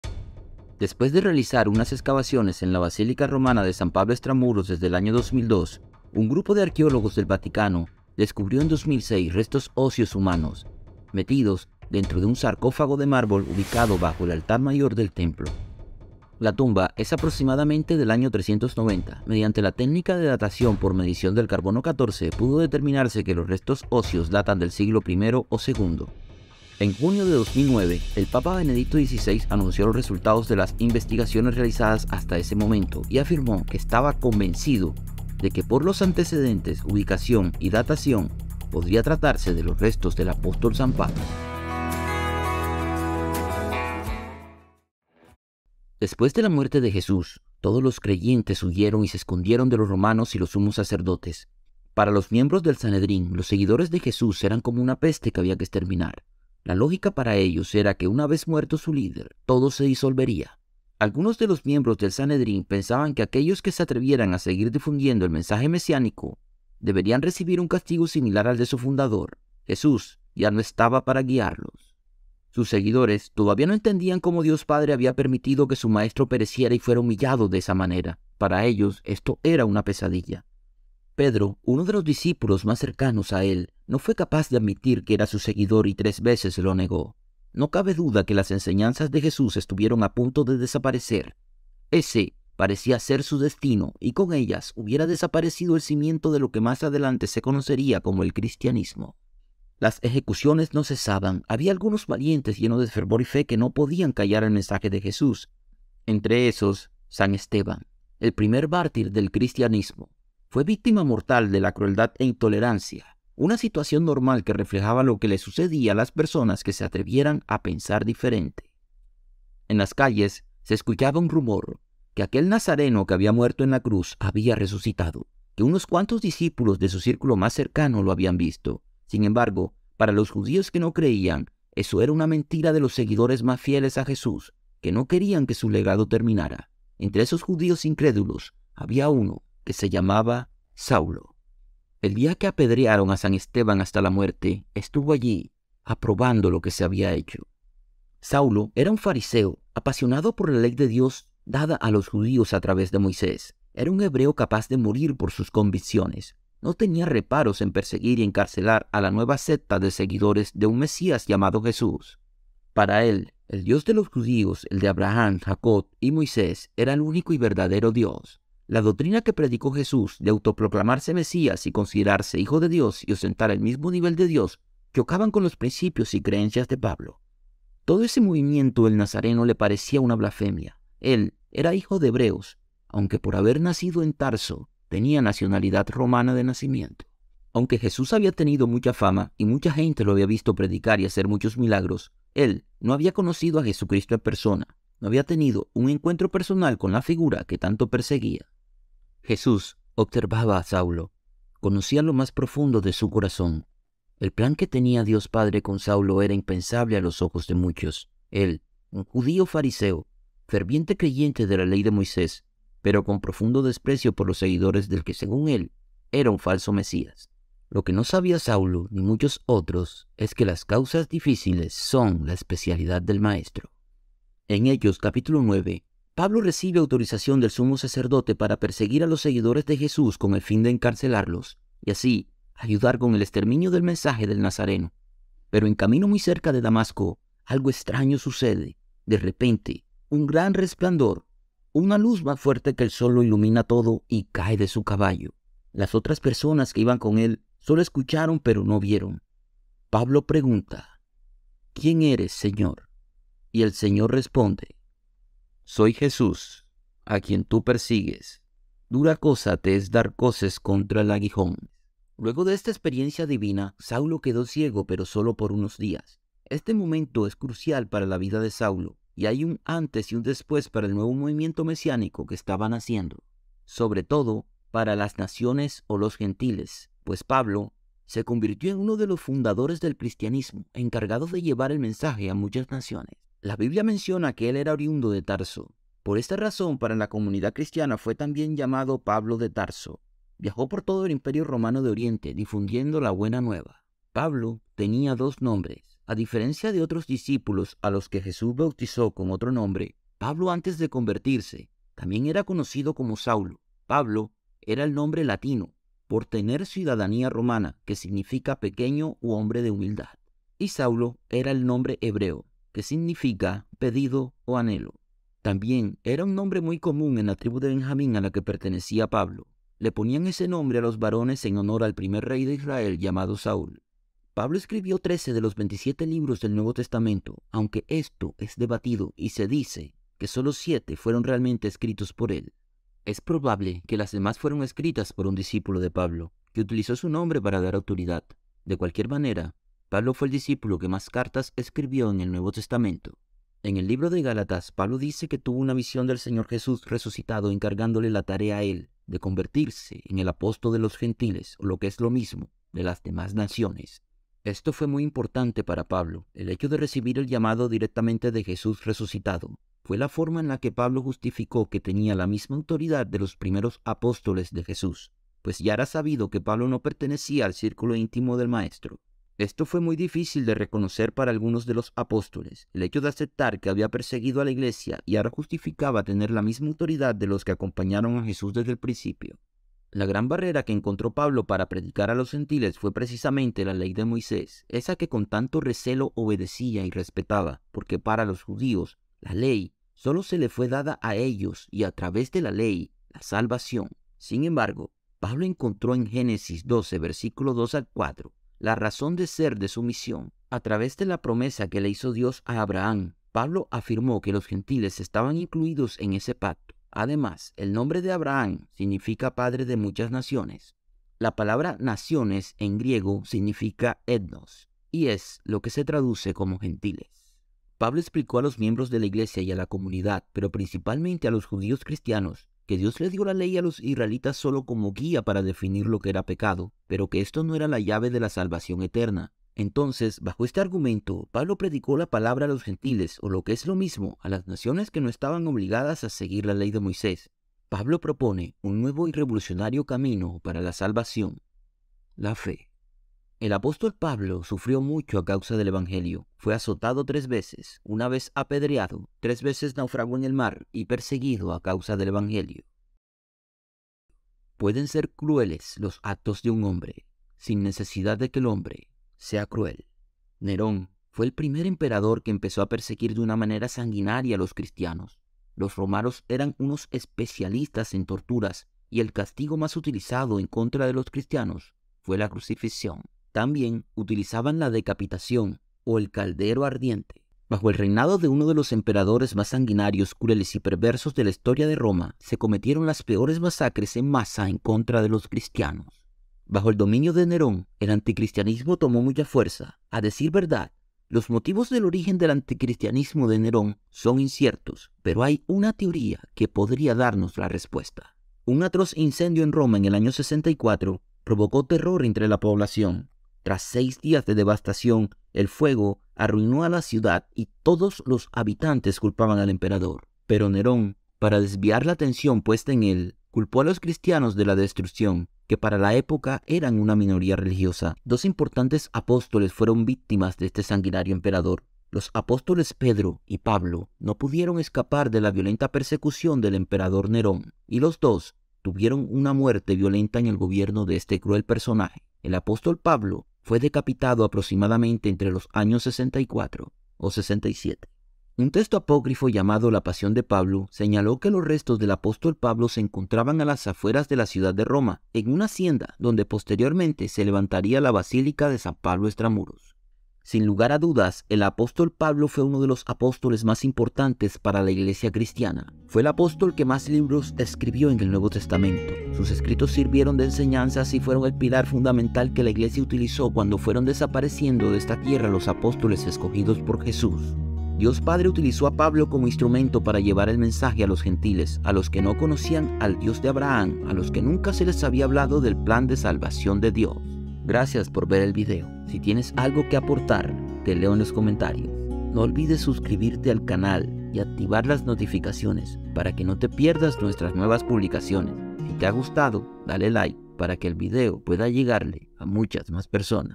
El apóstol san Pablo - DESCUBRE su Increíble historia.
En este video daremos un repaso de la vida del apóstol san Pablo, lo importante que fue para el cristianismo, su obra dejó un legado invaluable a la iglesia y a la historia  bíblica. También comentaremos la confirmación de sus restos anunciado por el entonces papa benedicto XVI ubicados en la basílica de san Pablo  extramuros.

De todos los apóstoles san Pablo fue quien más cartas escribió en el nuevo testamento, persiguió a los cristianos antes de convertirse, fue testigo de la muerte de san Esteban y encarcelaba a los cristianos.

Cuando se convirtió, predicó por toda roma, anunciando el evangelio de Jesucristo hasta su muerte.